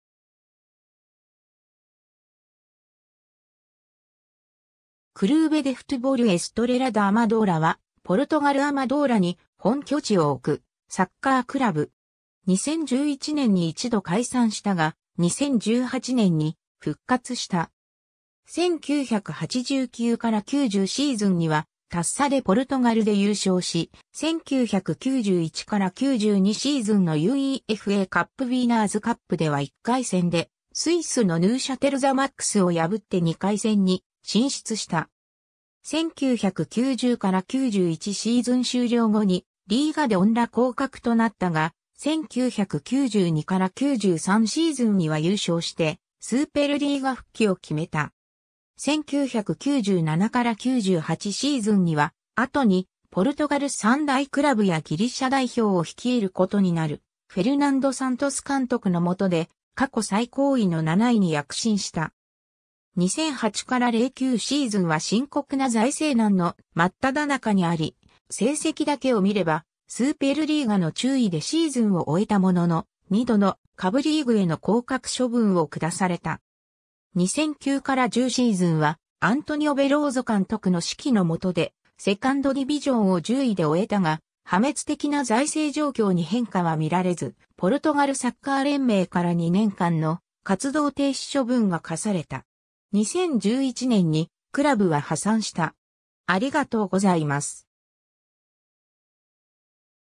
CFエストレラ・アマドーラ
クルーベ・デ・フトゥボル・エストレラ・ダ・アマドーラ（Clube de Futebol Estrela da Amadora ポルトガル語発音: [(ɨ)ʃˈtɾelɐ ðɐ ɐmɐˈðoɾɐ]）は、ポルトガル・アマドーラに本拠地を置くサッカークラブ。
2011年に一度解散したが、2018年に復活した。
 1989-90シーズンにはタッサ・デ・ポルトガルで優勝し、1991-92シーズンのUEFAカップウィナーズカップでは1回戦でスイスのヌーシャテル・ザマックスを破って2回戦に進出した。
1990-91シーズン終了後にリーガ・デ・オンラ（2部）降格となったが、1992-93シーズンには優勝してスーペル・リーガ（1部）復帰を決めた。
1997-98シーズンには、後にポルトガル3大クラブやギリシャ代表を率いることになるフェルナンド・サントス監督の下で、過去最高位の7位に躍進した。
 2008-09シーズンは深刻な財政難の真っただ中にあり、成績だけを見ればスーペル・リーガの中位でシーズンを終えたものの、2度の下部リーグへの降格処分を下された。
2009-10シーズンはアントニオ・ヴェローゾ監督の指揮の下でセカンド・ディビジョン（実質3部）を10位で終えたが、破滅的な財政状況に変化は見られず、ポルトガルサッカー連盟から2年間の活動停止処分が課された。
2011年にクラブは破産した。